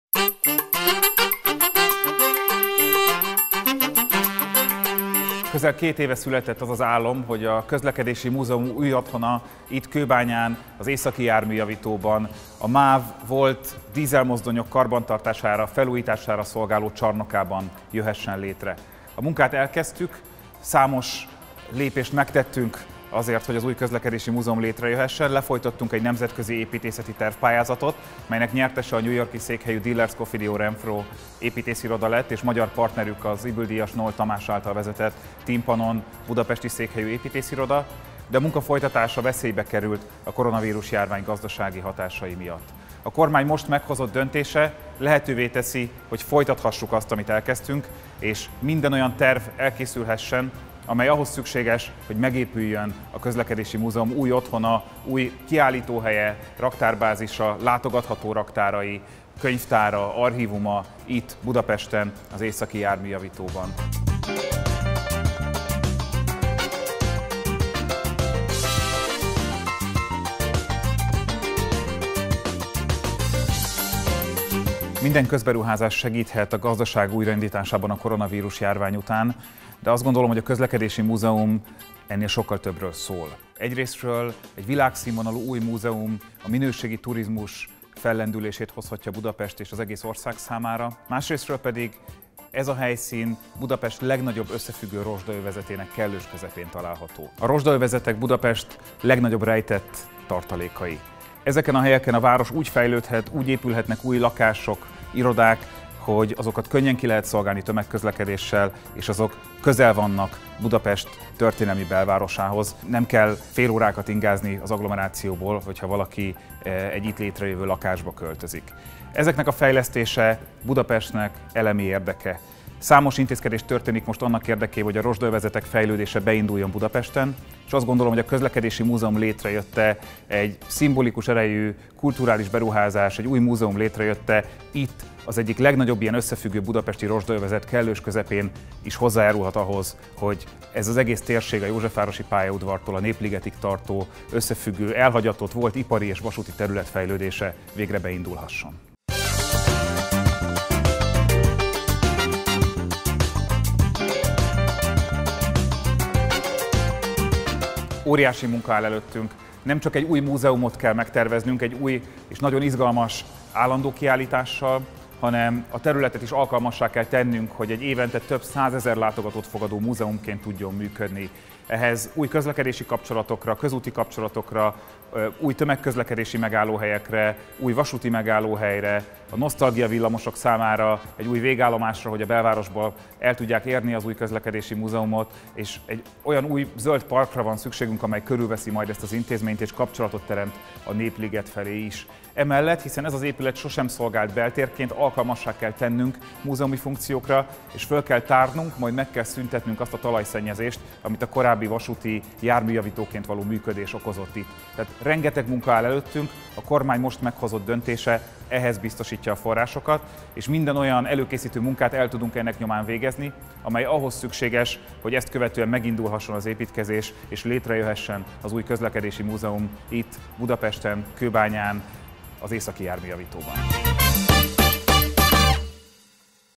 Döntött a Kormány: folytatódhat az új Közlekedési Múzeum tervezése
A Kormány tegnapi döntésével biztosította a forrásokat ahhoz, hogy folytathassuk az új Közlekedési Múzeum tervezését. A koronavírus-járvány az elmúlt néhány hónapban megkérdőjelezte a beruházás folytatásának lehetőségét, a döntés tehát fontos mérföldkő a Múzeum számára.

Az új Közlekedési Múzeum terveinek elkészítésére tavaly nemzetközi tervpályázaton választottuk ki a világhírű New York-i Diller Scofidio+Renfro építészirodát és magyar partnerét, az Ybl-díjas Noll Tamás vezette M-Teampannon építészirodát. A győztes koncepció nyomán azonnal megkezdődött a tartalmi munka, ami most újabb fázisába jutott, miután megkaptuk a Kormánytól a szükséges támogatást minden, a kivitelezés megkezdéséhez szükséges építészeti terv, előkészítő munka elvégzésére.

Ez a beruházás nem csak múzeumépítés, hanem átfogó városfejlesztés is, új zöldterületekkel, gyalogos kapcsolatokkal, tömegközlekedési fejlesztésekkel, melyek eredményeként óriási barnamezős területen teljesen új kulturális központ alakul majd ki Kőbányán, csatlakozva az Opera már elkészült gyönyörű új műhelyházához. A tervezési munka nem csak a múzeumépületre, hanem a környező parkra, az új állandó kiállításra, a Múzeum melletti új vasúti megállóra, közúti kapcsolatokra, új nosztalgiavillamosok számára készülő végállomásra és a környező kisebb épületek felújítására is kiterjed.